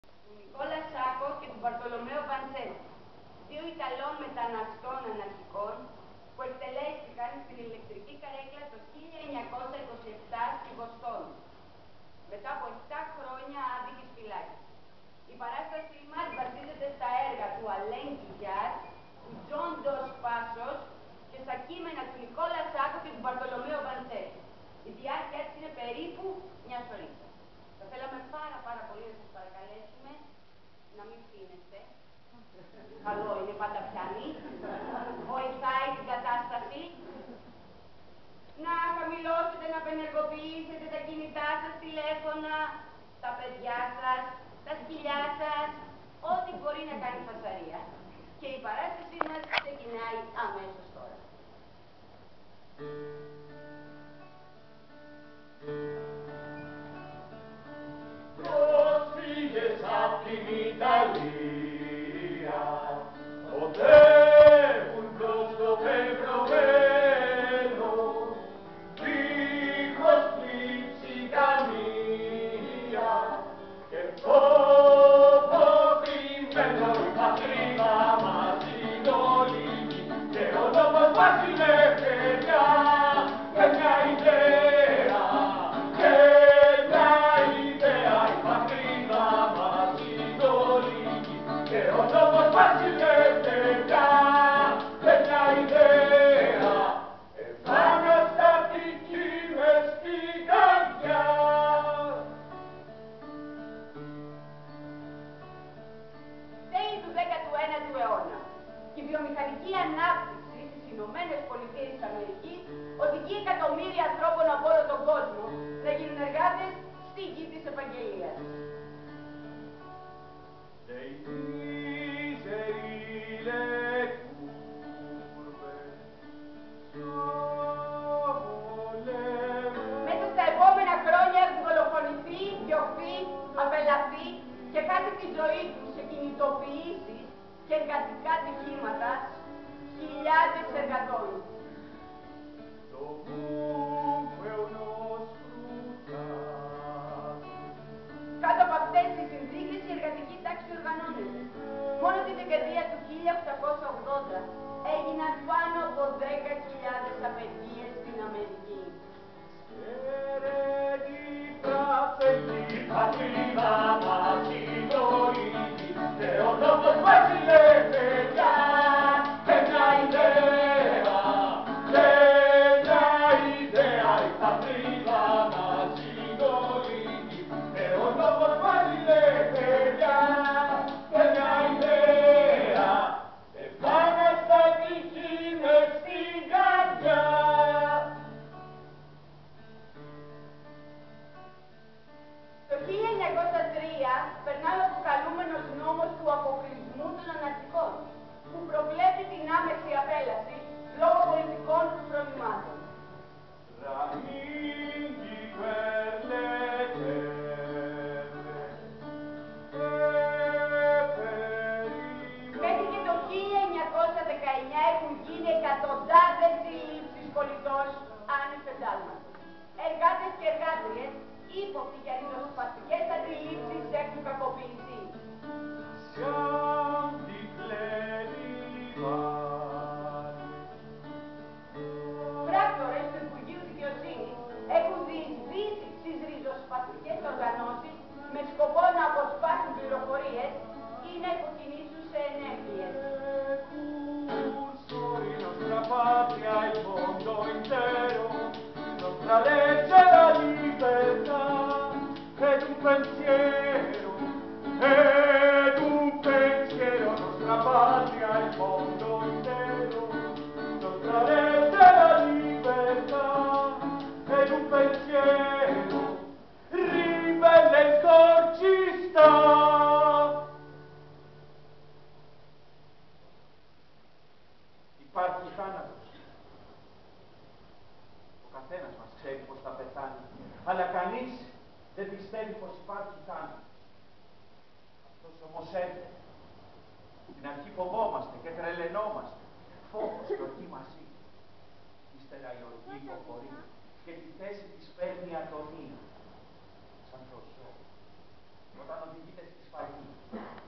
"Sacco & Vanzetti" by Tsiritsantsoules at Empros Theatre (10-3-2022)
"Sacco & Vanzetti", a play by the theatrical troupe "Tsiritsantsoules" took place at Empros Theatre, on the 10th of March in 2022. There is much narrating of historical events that followed after the accusation of Nicola Sacco and Bartolomeo Vanzetti. Both italian immigrants and anarchists. Poor workers, having been accused of a first-degree murder of a guard and a paymaster during the armed robbery of the Slater and Morrill Shoe Company in Braintree, a place in Massachusetts in 1920, they would be executed in the electric chair at Charlestown State Prison seven years later as it happened. The playscript is based on work of Alain Guyard, John Dos Passos, Nicola Sacco & Bartolomeo Vanzetti. Director: Marinos Mouzakis. Starring in: Ntina Mavridou, Yannis Adrimis, Marinos Mouzakis.